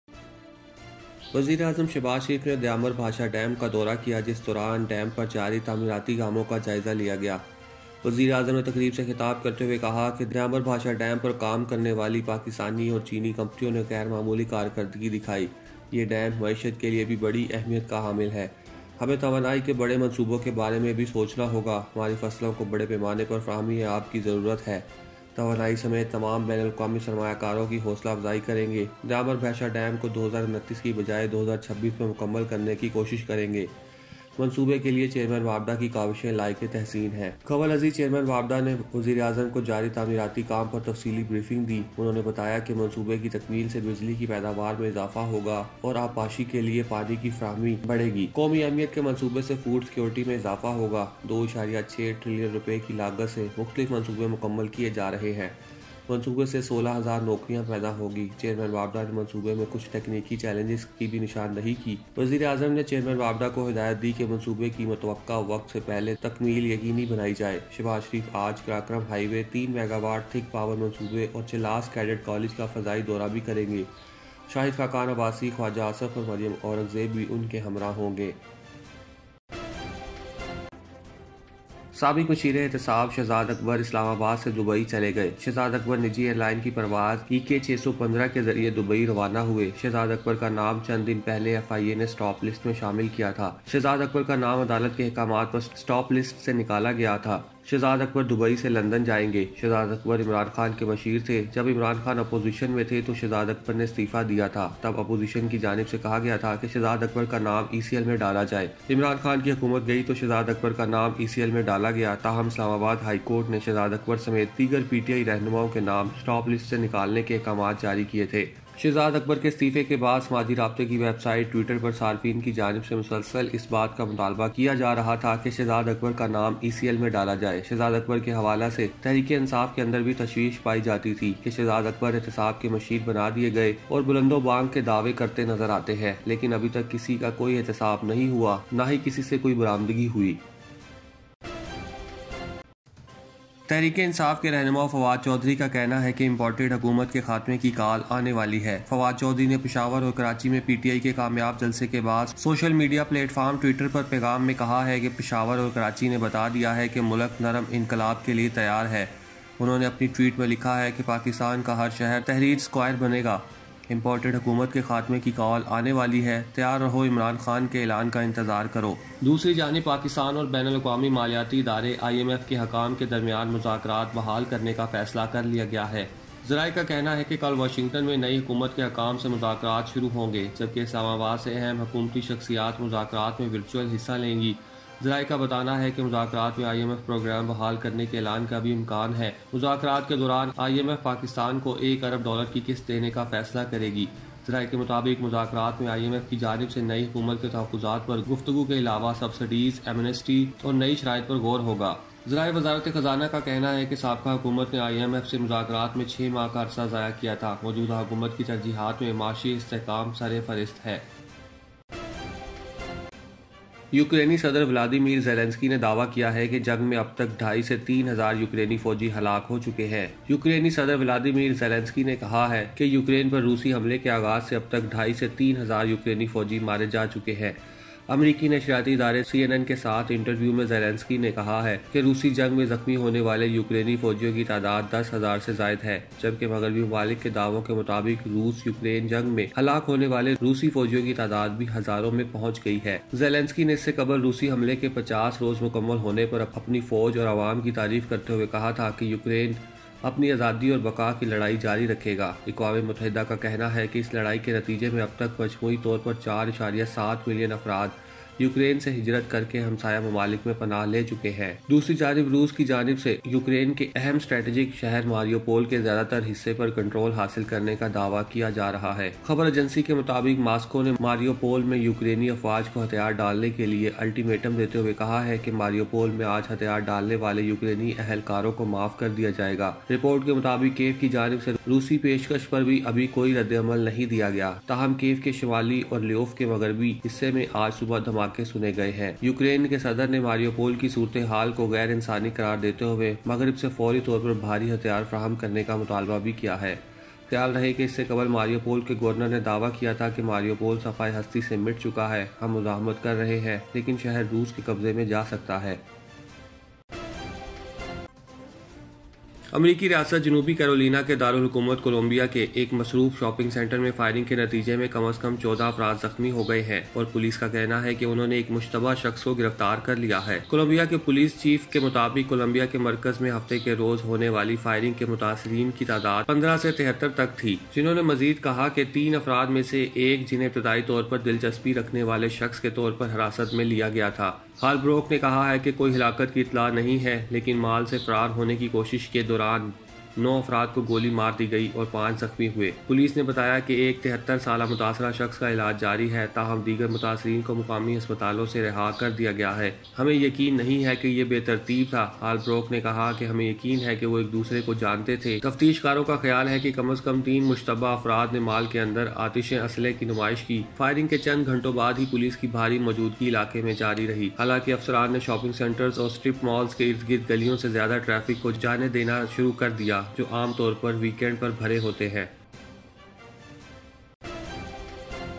Pak Watan News Buliten - Call for an end to the imported government is coming Said Fawad Chaudhary
Pak Watan News Buliten